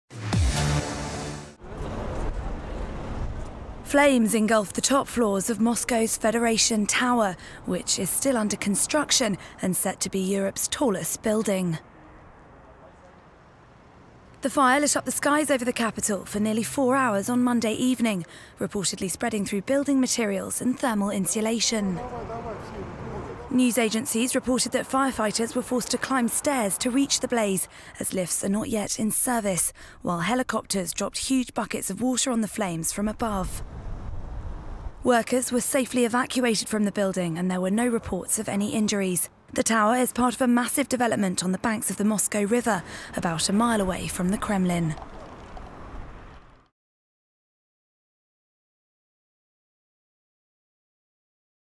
Flames engulf Moscow skyscraper, the Federation Tower
A blaze has engulfed the top floors of Moscow's Federation Tower, which is set to be Europe's tallest building once completed. Report by Katie Lamborn.